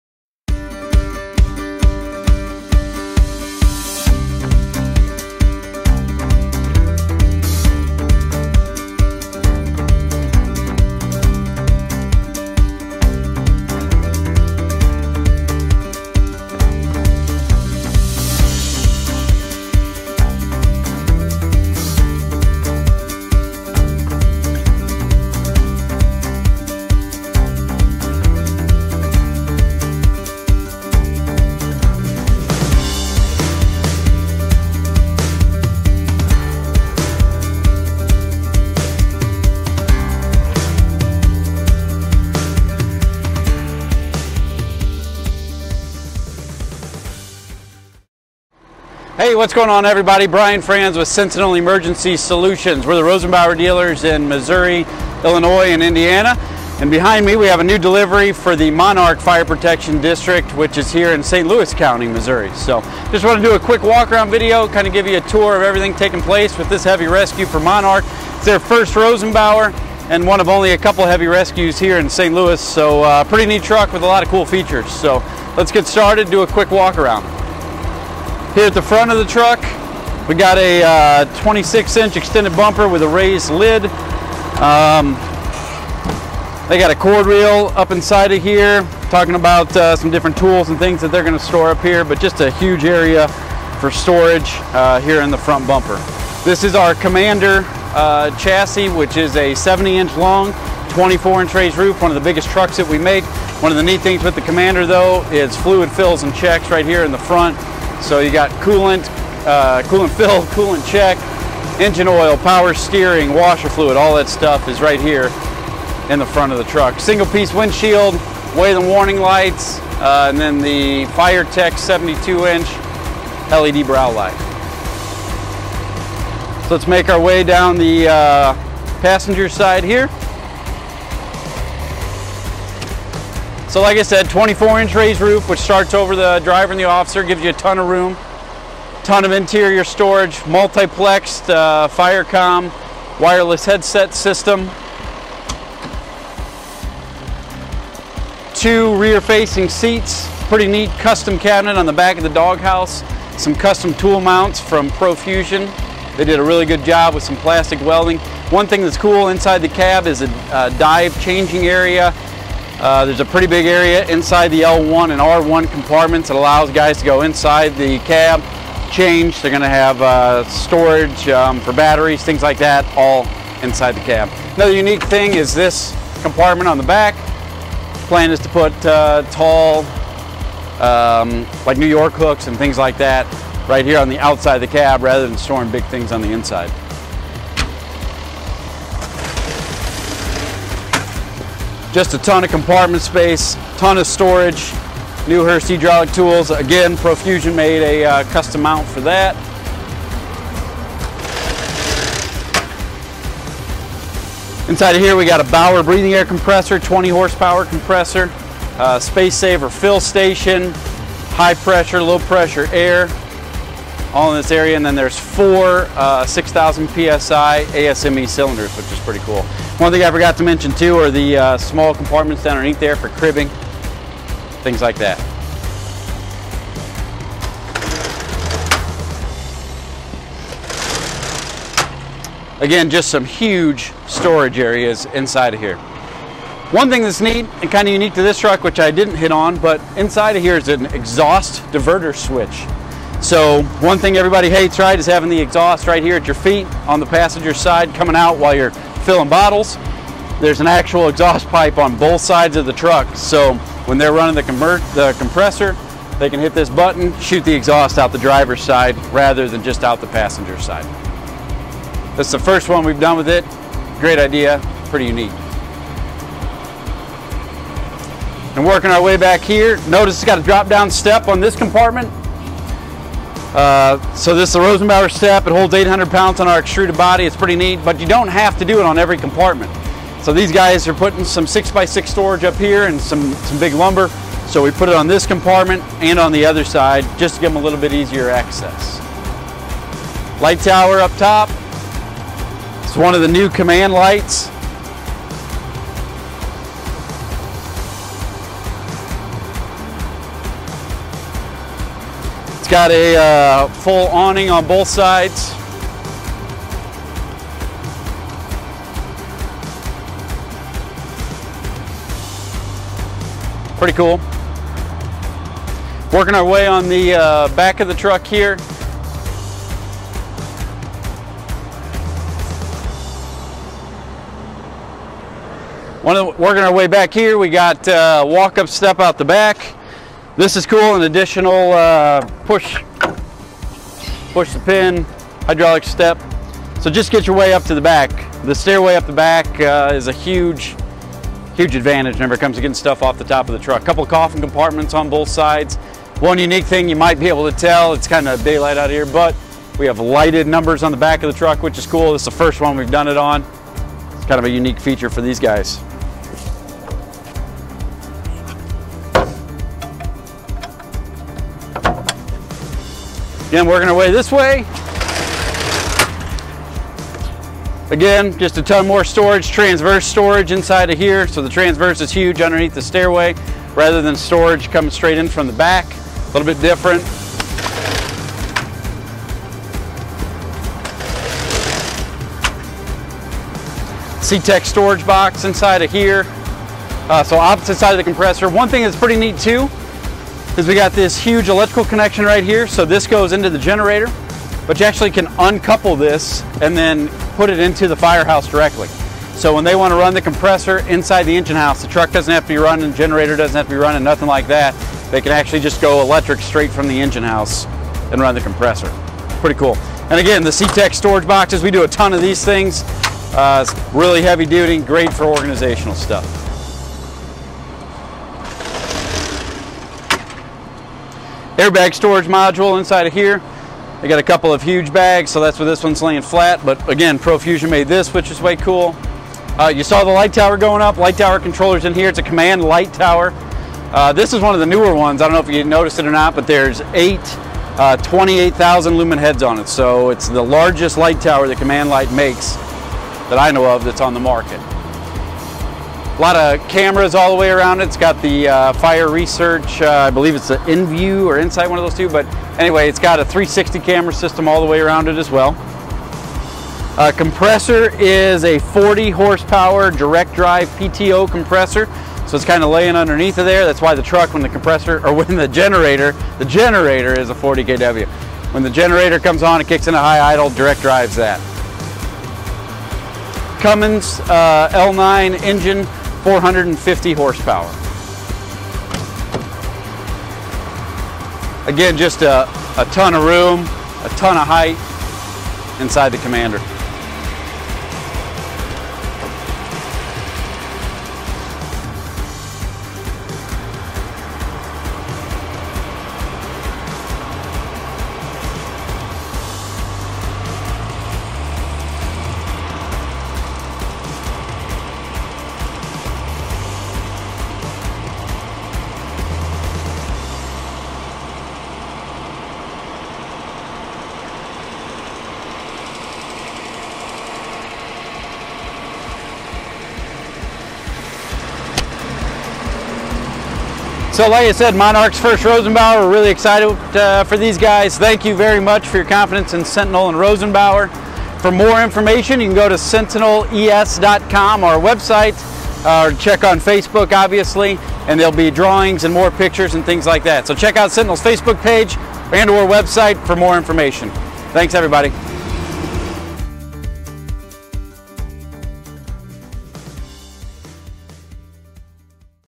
Monarch FPD (Chesterfield, MO) Rosenbauer Heavy Rescue Walk Around
Monarch FPD took delivery of this Rosenbauer Heavy Rescue in February 2021. SENTINEL Apparatus Specialist, Brian Franz, takes us on a detailed walk around.

Sentinel Emergency Solutions
2900 Telegraph Rd